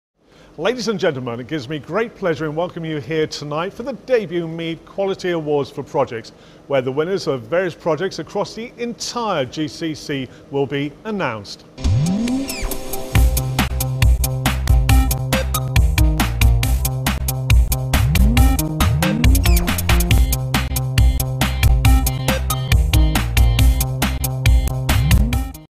Phil Blizzard introducing the MEED Quality Awards for Projects
Broadcaster Phil Blizzard in action at the MEED Quality Awards for Projects in the GCC region. The event was a glittering gala occasion held at the Ritz Carlton, DIFC in Dubai